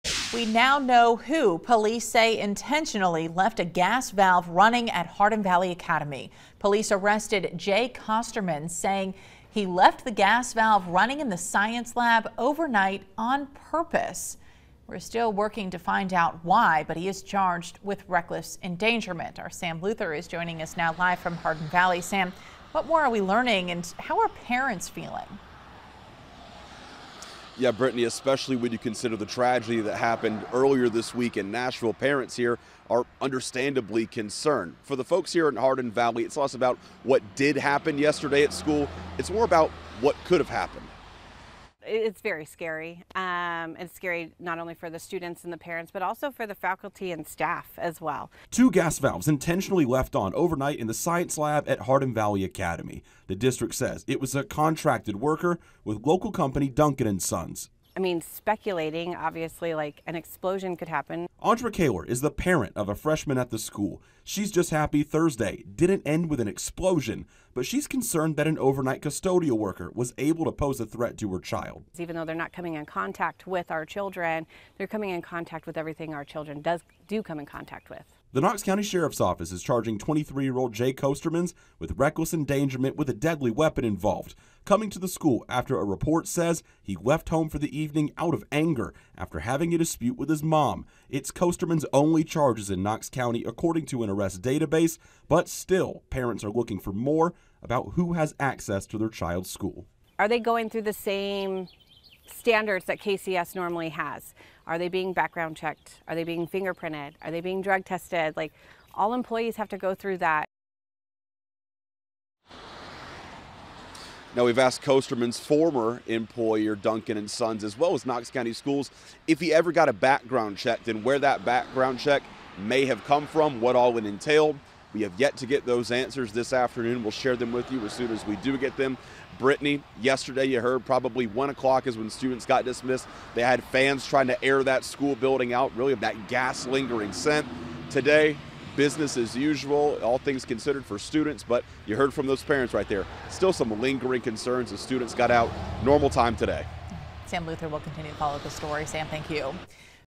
Employee accused of intentionally leaving gas valves on in HVA classroom identified
Hardin Valley Academy dismissed students early after two gas valves in a science lab were left running overnight, causing disruptions to learning, Knox County Schools officials told WVLT News Thursday.